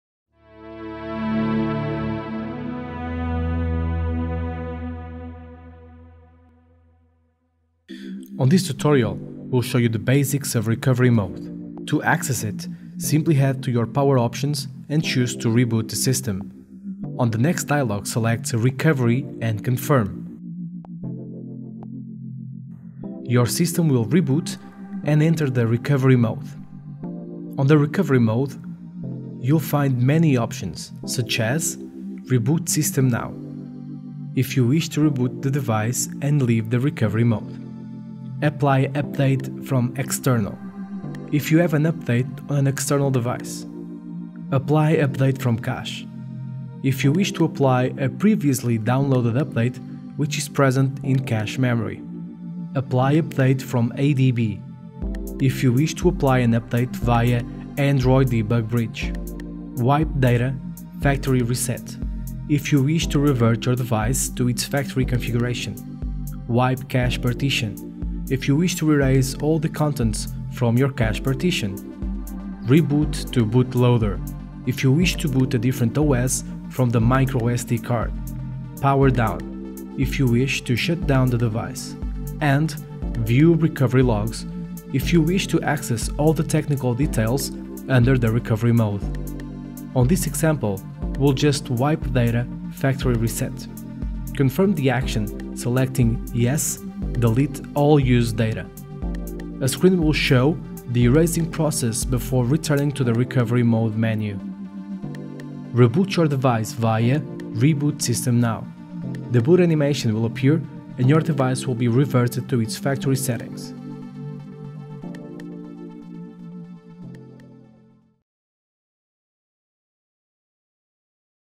Recovery Mode - WeOS 1.0 [WeTek Tutorials]
Learn the basic options when booting on Recovery Mode